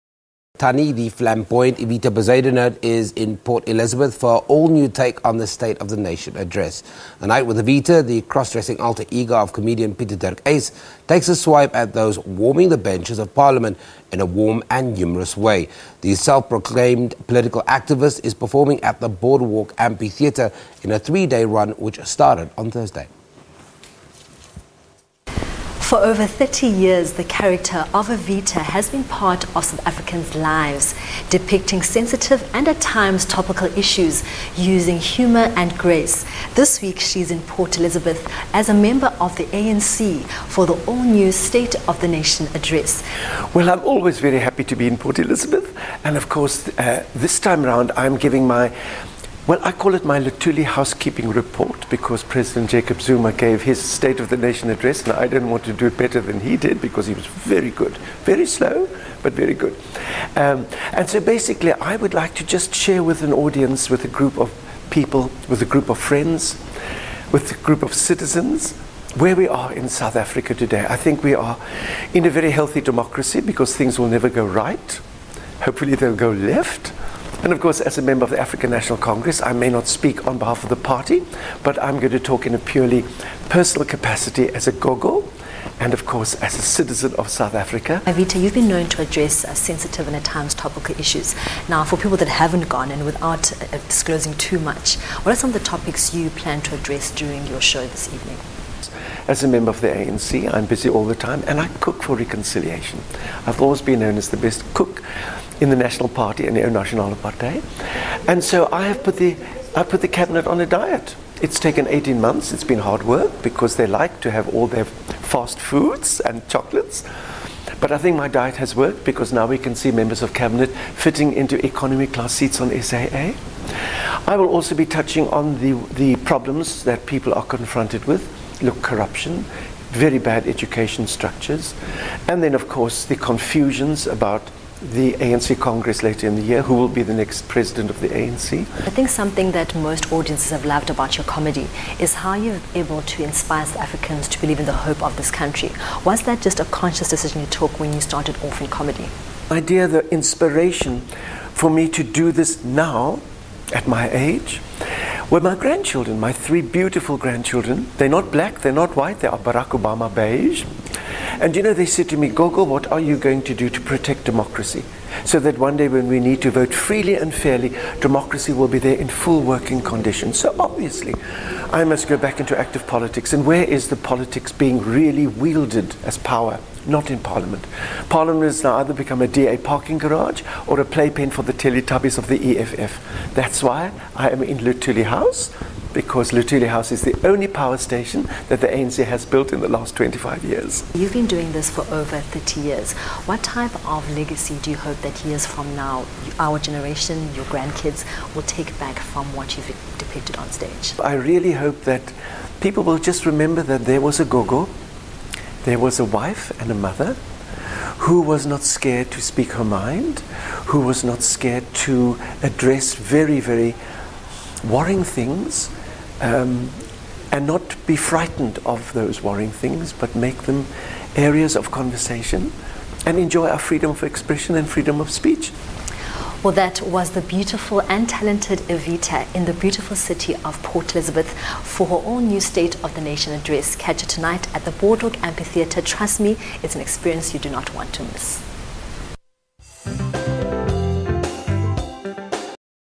Evita Bezuidenhout in PE for her "State of the Nation Address"
South Africa's all time favourite tannie, the flamboyant Evita Bezuidenhout is in Port Elizabeth for her all new take on the "State of the Nation Address". A Night With Evita,  the cross-dressing alter ego of comedian Pieter-Dirk Uys takes a swipe at those warming the benches of parliament in a warm and humorous way. The self-proclaimed political activist is performing at the Boardwalk Amphitheatre in a three-day run which started on Thursday.